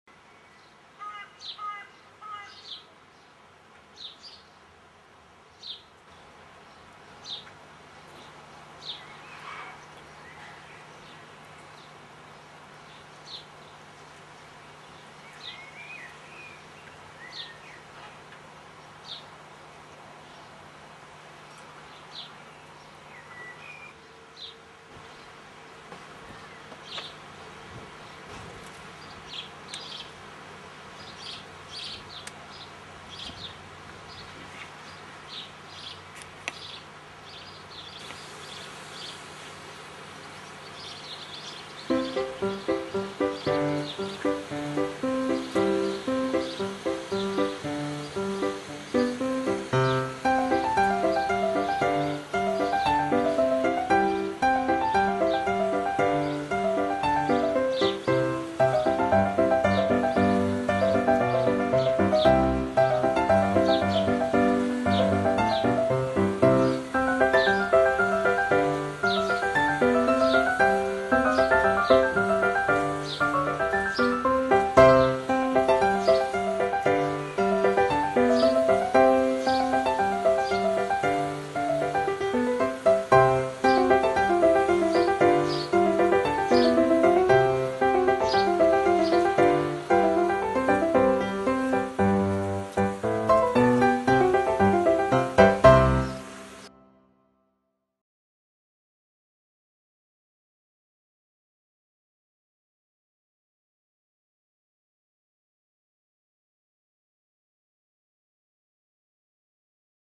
Garden Birds June 2013
Some of the wild birds that visit our garden